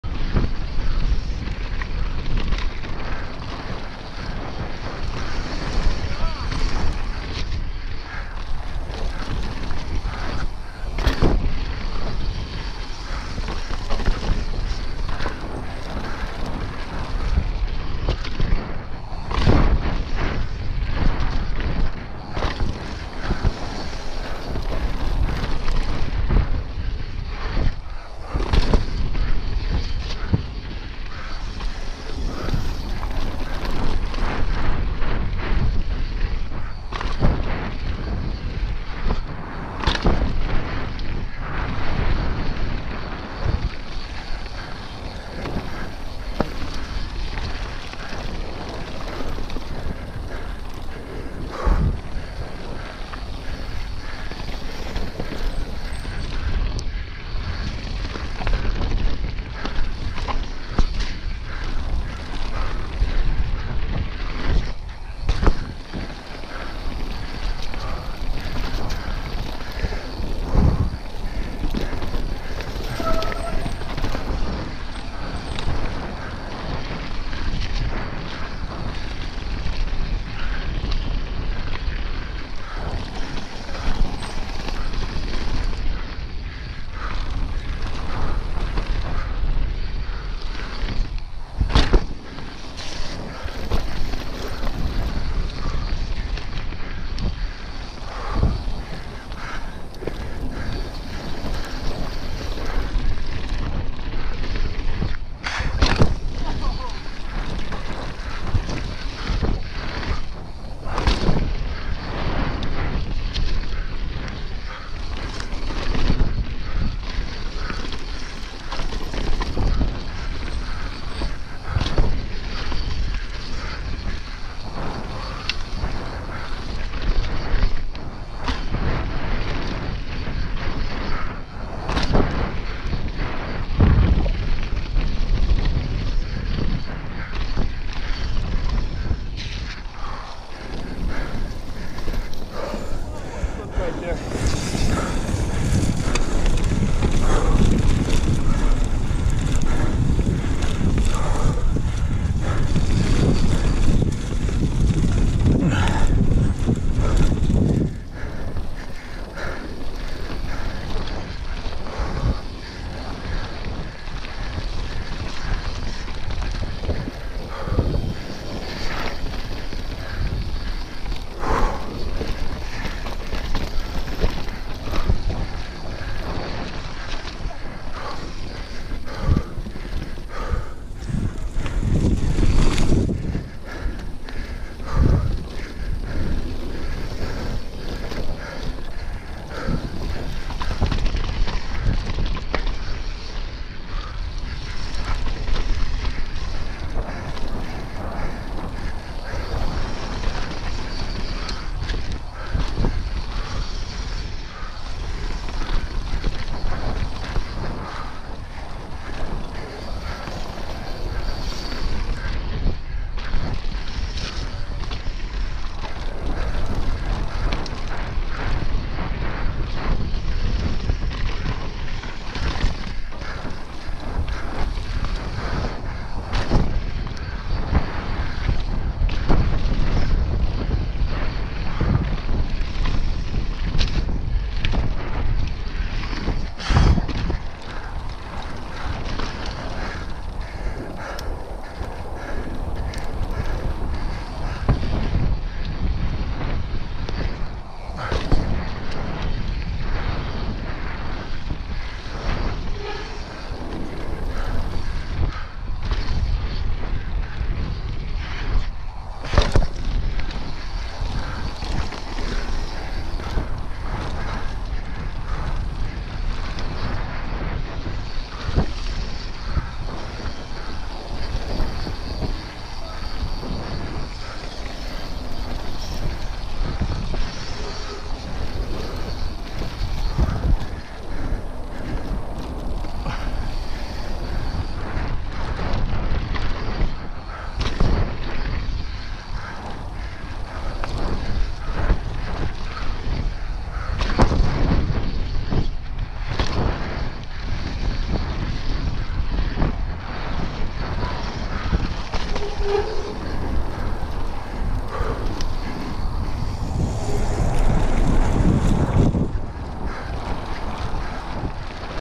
Jeff Rippin the Yeti SB 150
Jeff W riding the “Freewheel “ trail on my Yeti for the first time. I think he needs one for himself, he deserves it.